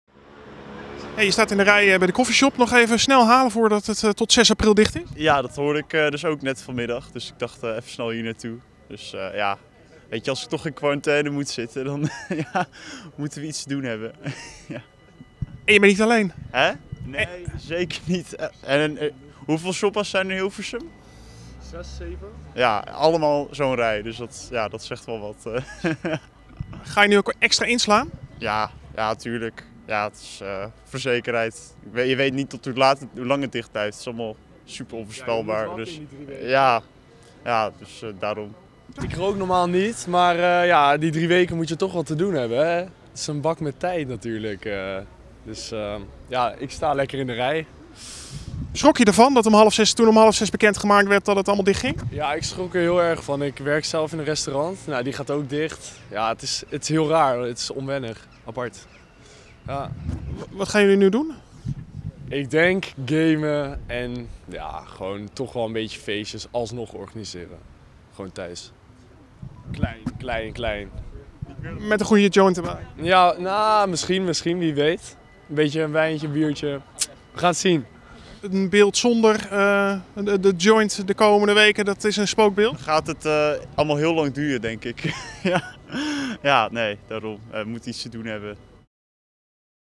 Wiet hamsteren bij de coffeeshops door coronavirus
Een rij van tientallen meters vandaag voor één van de coffeeshops in Hilversum. Zo vlak voor de sluiting van drie weken willen klanten nog snel een voorraad inslaan.